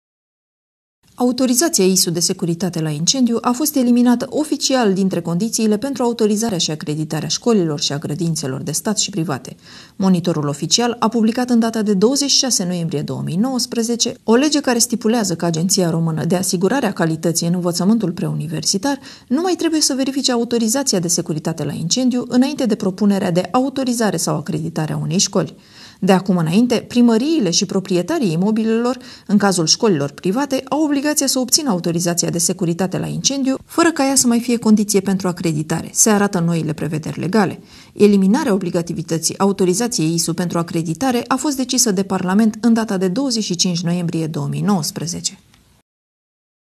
Știri Locale - Autorizația ISU, eliminată de la acreditarea școlilor
Securitate la incendiu a fost eliminată oficial dintre condițiile pentru autorizarea și acreditarea școlilor și grădinițelor.